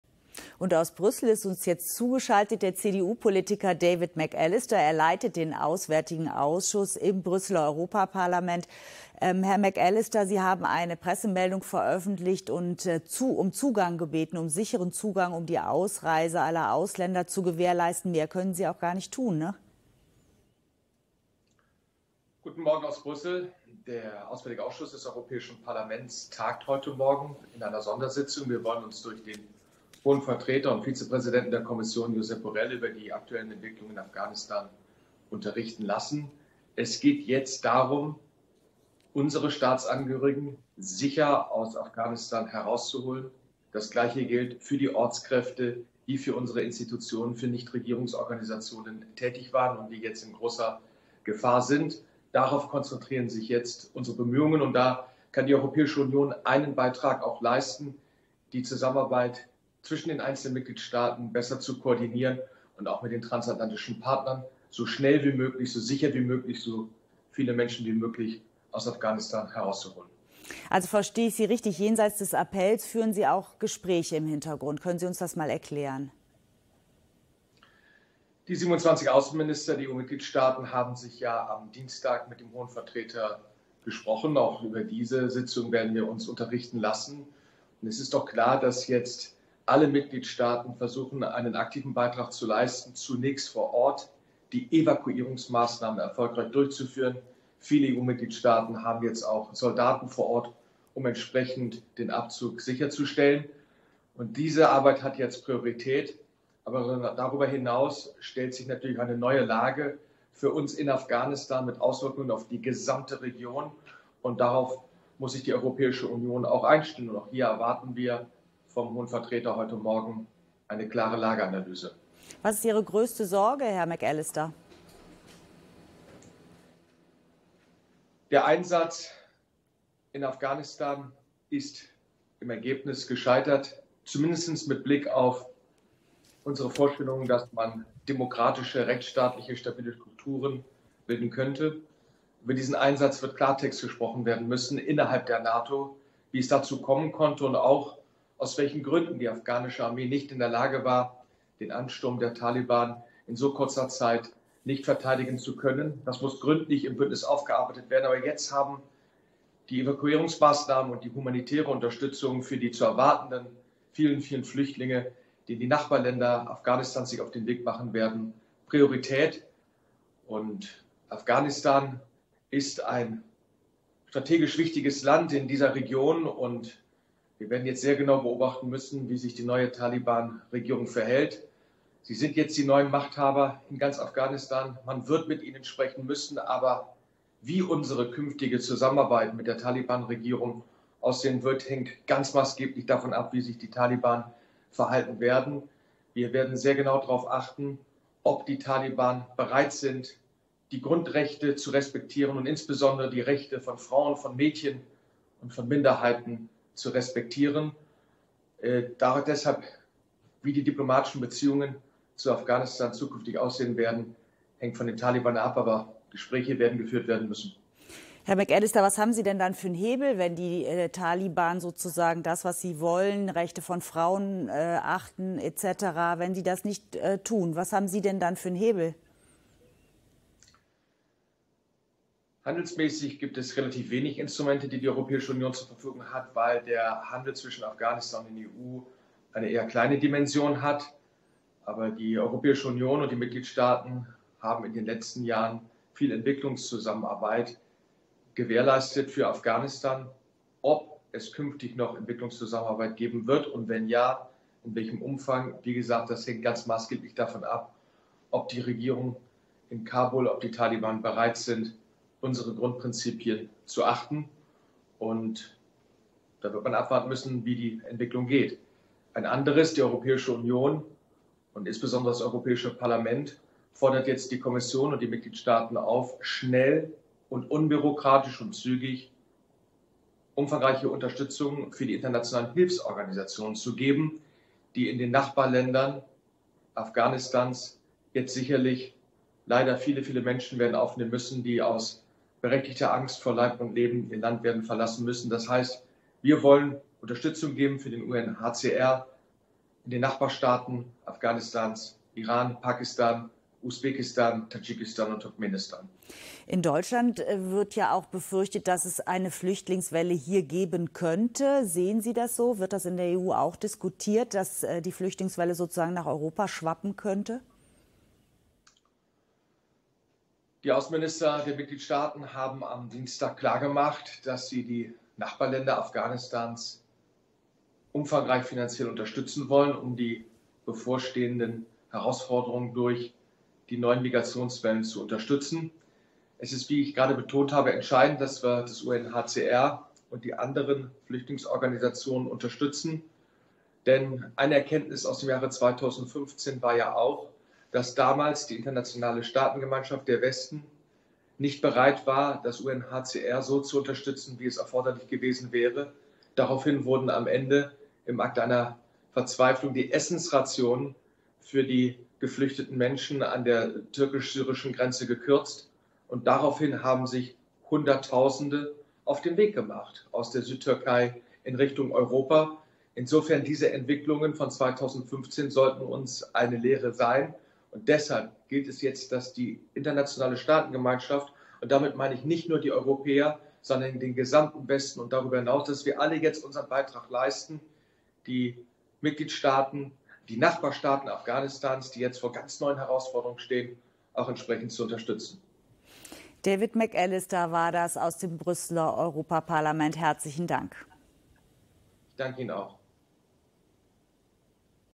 David McAllister zur aktuellen Lage in Afghanistan
Schaltgespräch mit David McAllister (CDU/EVP-Fraktion und Vorsitzender des Auswärtigen Ausschusses im EU-Parlament) anlässlich der Sondersitzung des Auswärtigen Ausschusses im EU-Parlament zur aktuellen Lage in Afghanistan.

Der Vorsitzende des Auswärtigen Ausschusses im EU-Parlament, David McAllister (CDU/EVP), hat die weitere Entwicklungszusammenarbeit der EU-Staaten mit Afghanistan davon abhängig gemacht, dass die Taliban westliche Grundprinzipien achten. McAllister nannte im phoenix-Interview insbesondere „die Rechte von Frauen und Mädchen sowie von Minderheiten“. Das Europäische Parlament habe die EU-Kommission und die Mitgliedstaaten darüber hinaus aufgefordert, jetzt die internationalen Hilfsorganisationen in den Nachbarländern Afghanistans zu unterstützen, die vielen Menschen werden helfen müssen. „Wir wollen dem UNHCR Unterstützung geben und den Nachbarstaaten Iran, Pakistan, Usbekistan, Tadschikistan und Turkmenistan“, sagte McAllister. Dies hätten die Außenminister der Mitgliedstaaten auch bereits zugesagt.

McAllister erinnerte daran, dass die Flüchtlingswelle 2015 unter anderem auch dadurch ausgelöst worden sei, dass dem UN-Flüchtlingskommissariat UNHCR nicht in dem Maße geholfen worden sei, wie es notwendig gewesen wäre. „Daraufhin wurden am Ende im Akt einer Verzweiflung die Essenrationen für die geflüchteten Menschen an der türkisch-syrischen Grenze gekürzt und daraufhin haben sich Hunderttausende auf den Weg gemacht, aus der Südtürkei in Richtung Europa.“ Die Erkenntnis daraus müsse sein, dass die EU und der gesamte Westen die Nachbarstaaten Afghanistans, die jetzt vor großen Herausforderungen stünden, entsprechend unterstützten, so der Europaabgeordnete.